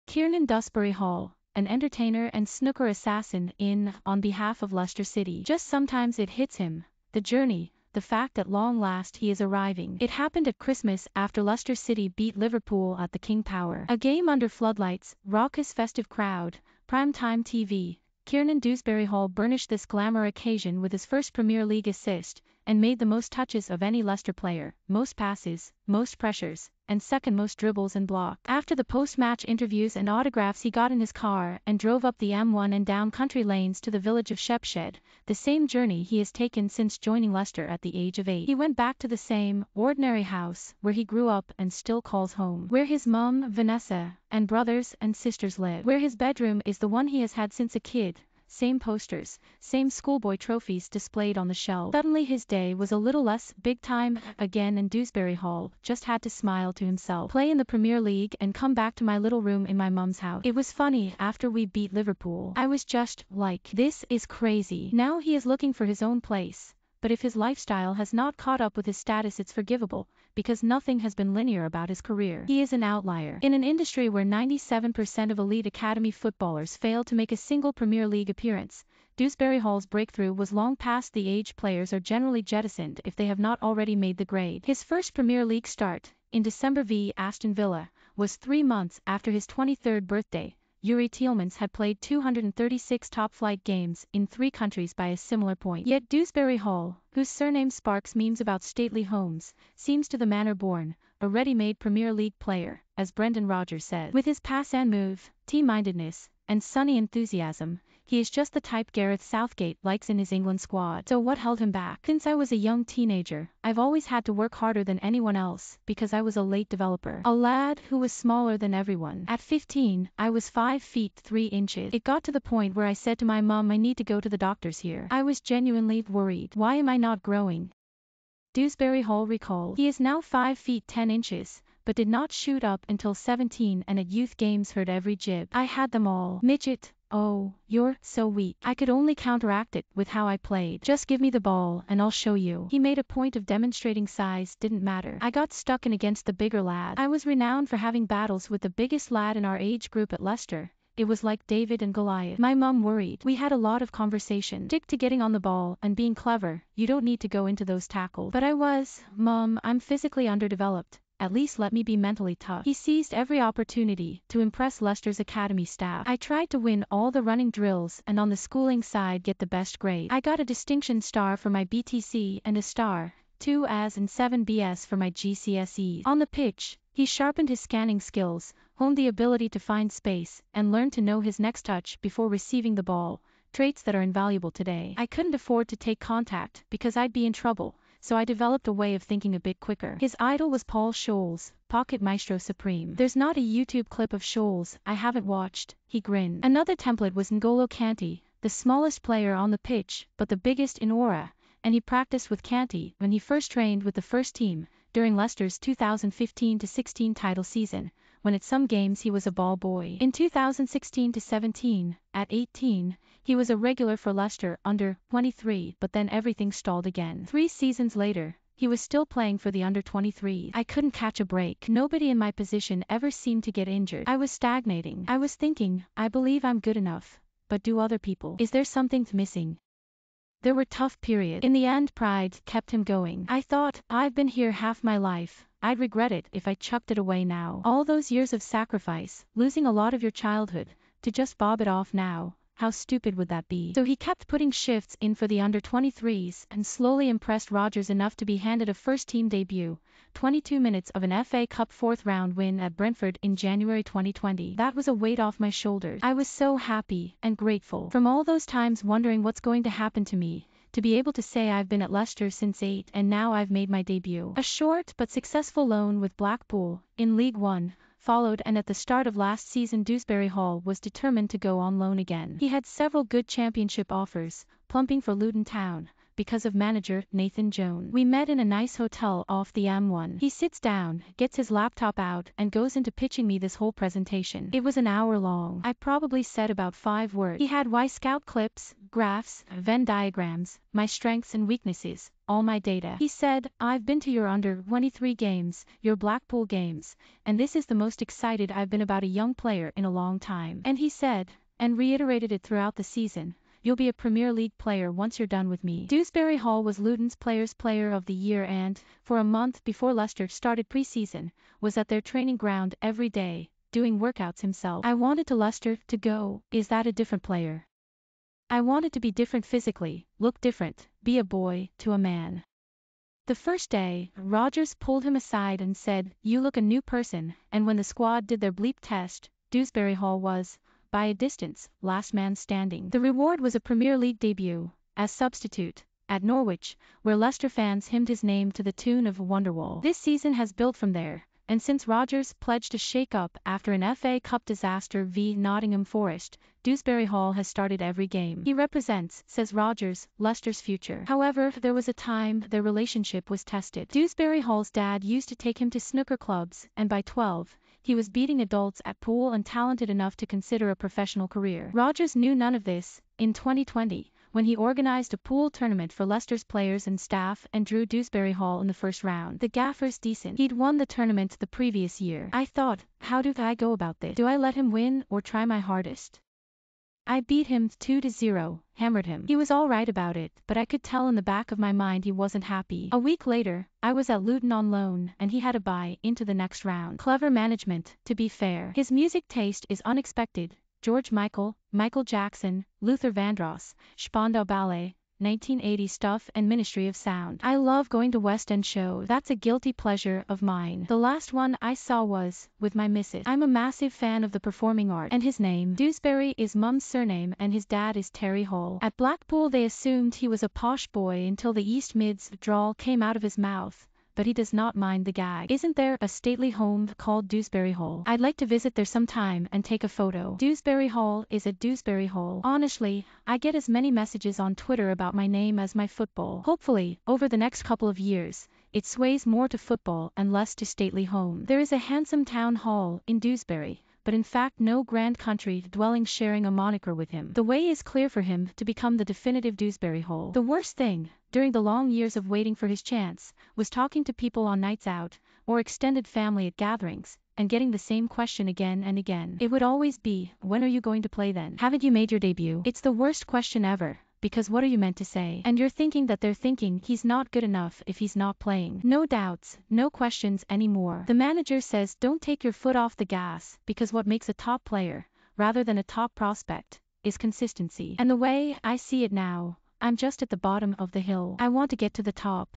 Kiernan Dusbury Hall: an entertainer and snooker assassin in "on behalf of Leicester City"
There’s not a YouTube clip of Scholes I haven’t watched I was 5’3. It got to the point where I said to my mum I need to go to the doctors here. I was genuinely worried. Why am I not growing I couldn’t catch a break. Nobody in my position  ever seemed to get injured. I was stagnating. I was thinking ‘I believe I’m good enough — but do other people to be able to say I’ve been at Leicester since eight and now I’ve made my debut during Leicester’s 2015-16 title season — when at some games he was a ball boy. In 2016-17 I’ve always had to work harder than anyone else because I was a late developer. A lad who was smaller than everyone. At 15 a ready-made Premier League player you’re so weak’. I could only counteract it with how I played. ‘Just give me the ball and I’ll show you.’ I got stuck in against the bigger lads. I was renowned for having battles with the biggest lad in our age group at Leicester Since I was a young teenager so I developed a way of thinking a bit quicker. Play in the Premier League and come back to my little room in my mum’s house